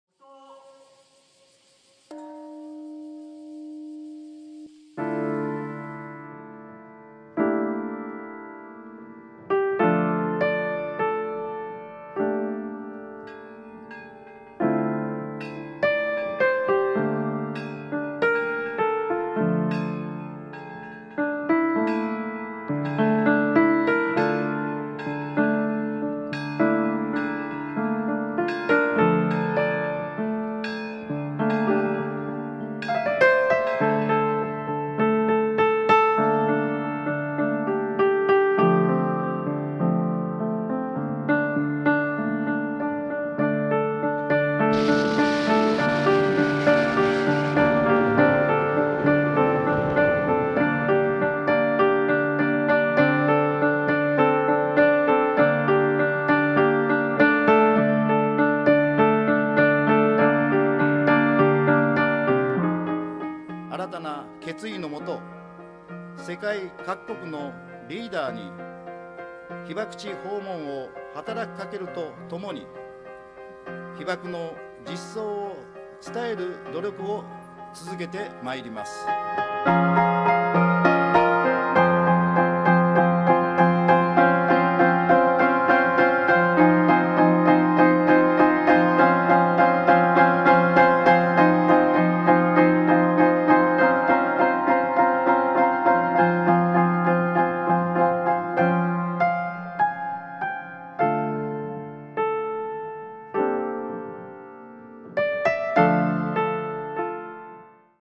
Ядерной бомбардировке Японии 71 год
В Японии сегодня вспоминают ужасные события августа 1945 года – тогда США сбросили ядерные бомбы на города Хиросима и Нагасаки. Погибли более 200 тысяч человек